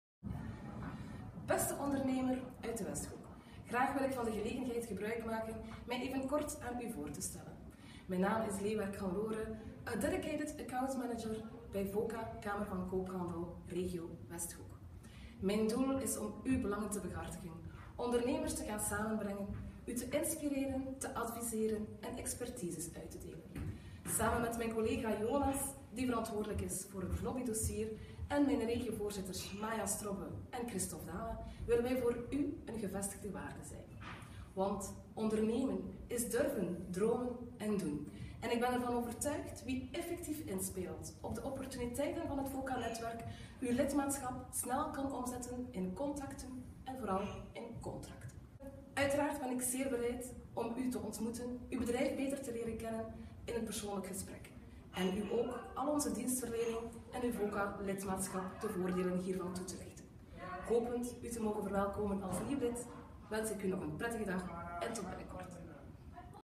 Hannelore Leeuwerck - Accountmanager Westhoek
Hannelore Leeuwerck is accountmanager bij Voka West-Vlaanderen voor de regio Westhoek.